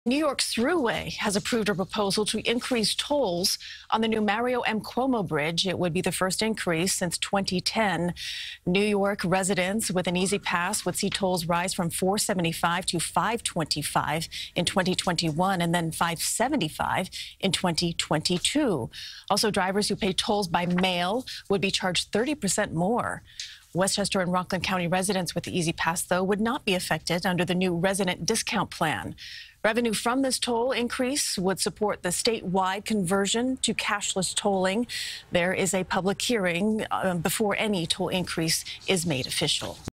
Toll Hike Approved On Mario Cuomo Bridge
New York officials have approved a toll hike on the Mario M. Cuomo bridge.